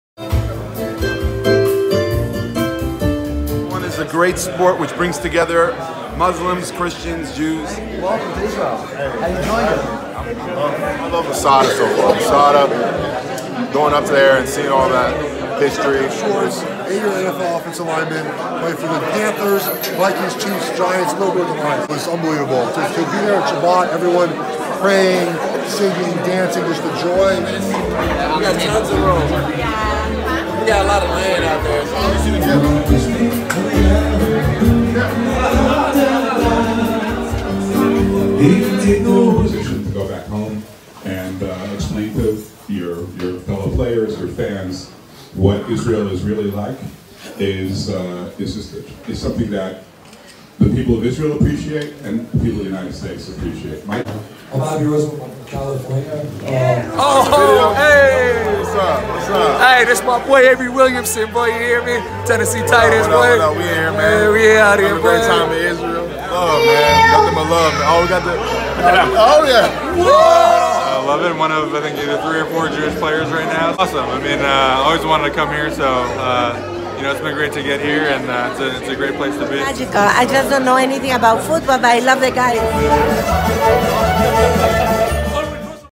A Night with NFL in Jerusalem
A night in Jerusalem with seven NFL legends and friends!